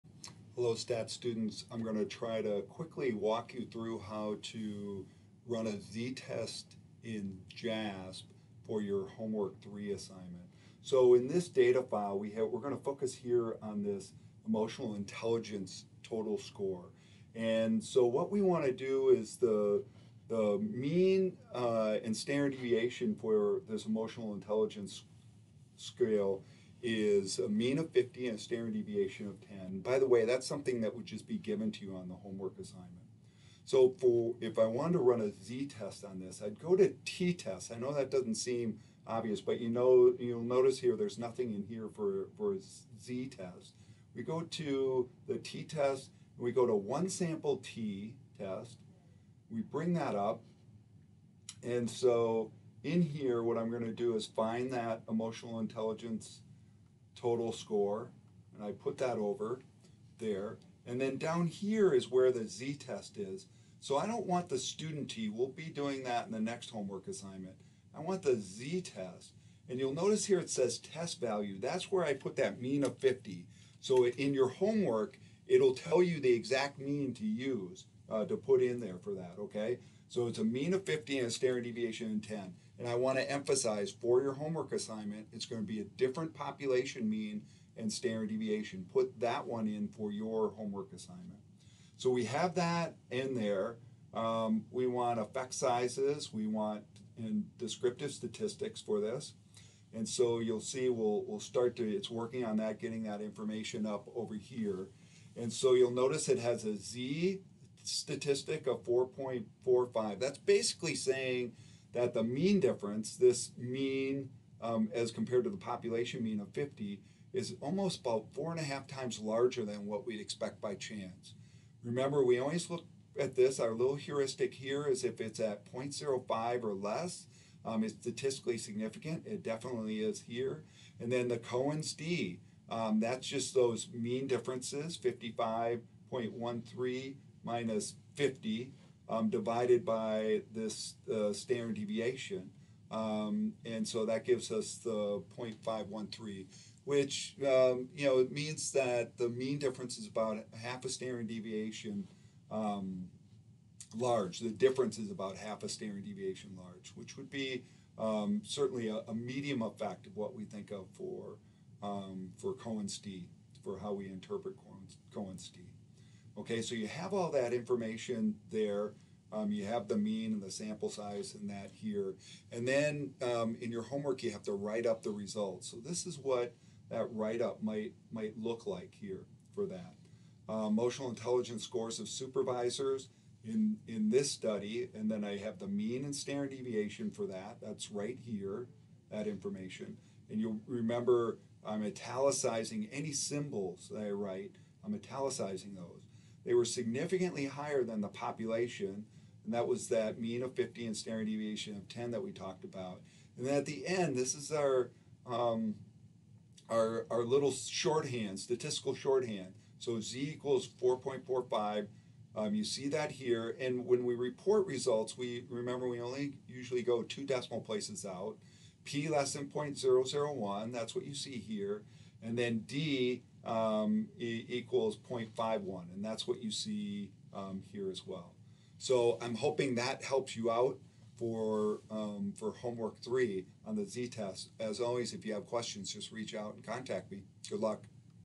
z-test in JASP
Demonstration of how to do a z-test in JASP and how to write up the results in APA style.